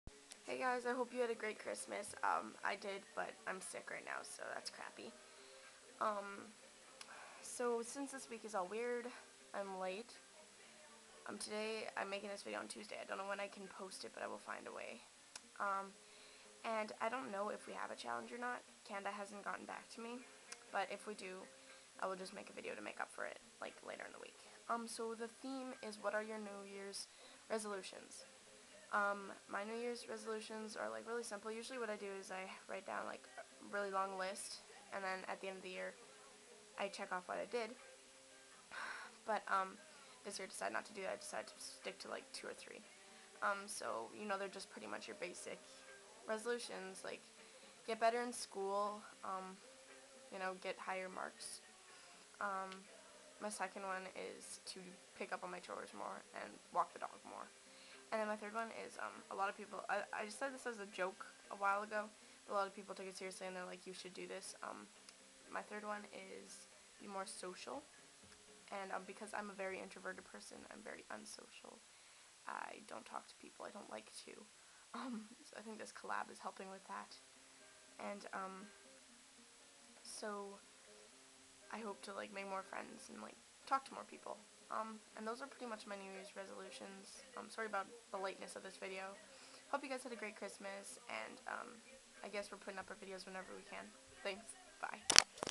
Monday Is Late
So, there is only a theme this week, it is "What are your New Year's resolutions?"
All will be normal next week, hopefully. :)